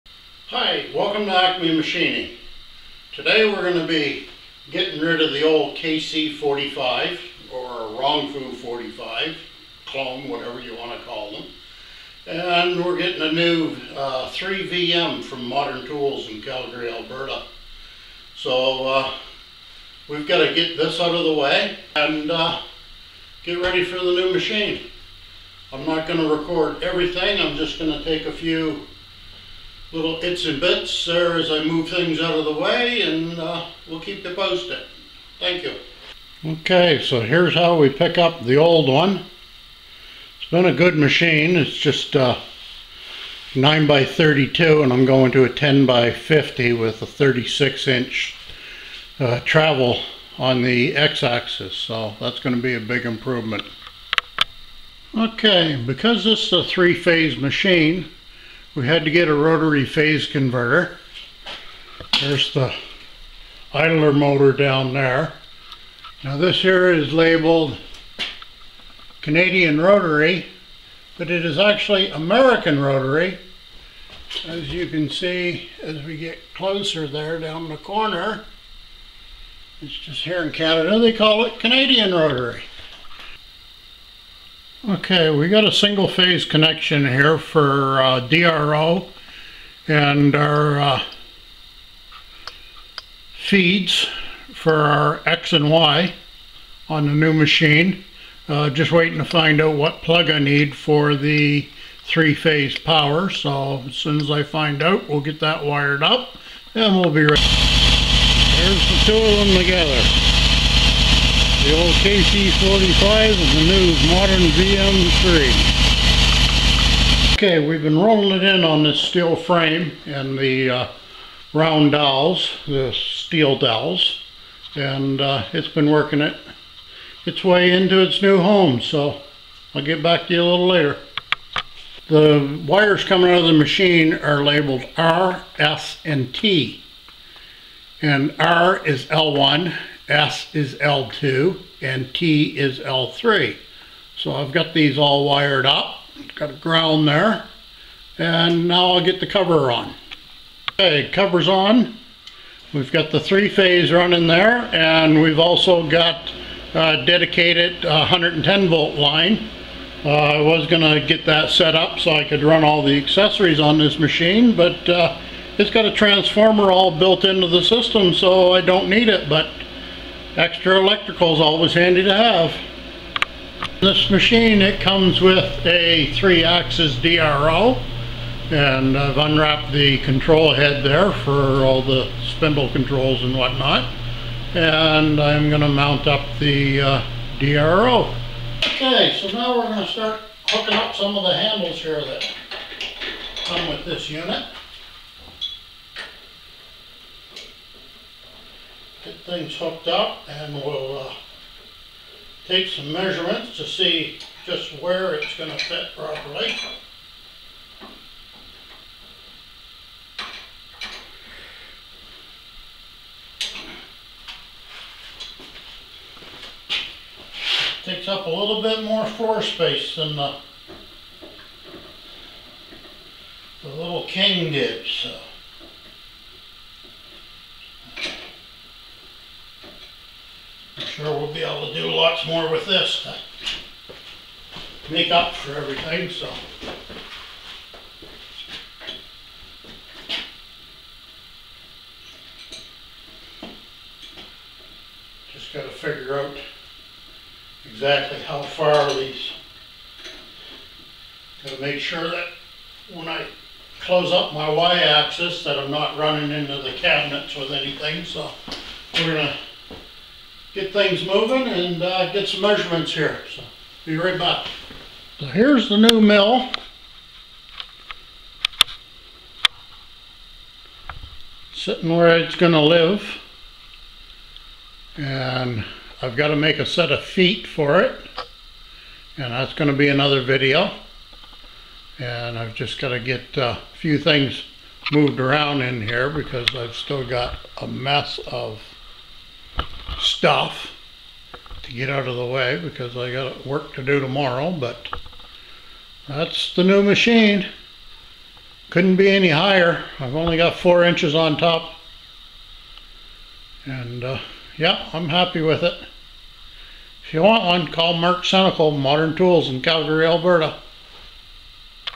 New 3VM Milling Machine
Upgrade to full size milling machine, a 3VM from Modern Tools in Calgary Alberta. Looking forward to the added capabilities of a knee mill with 3 axis digital readout.